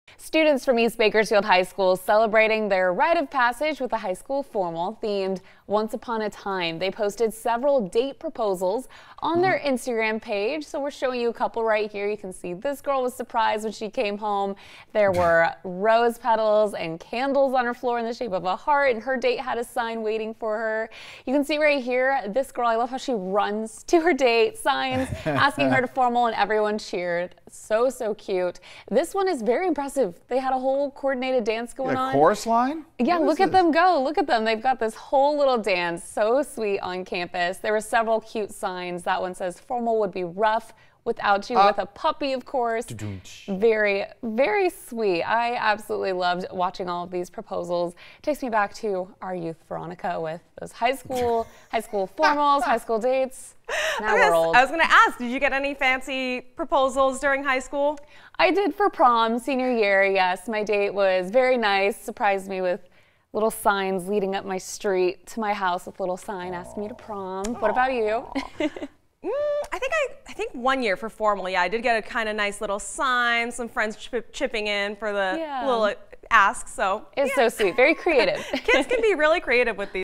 EBHS formal date proposals
EBHS students got creative in asking their dates to this past weekend's winter formal - check out some of our favorite date "proposals."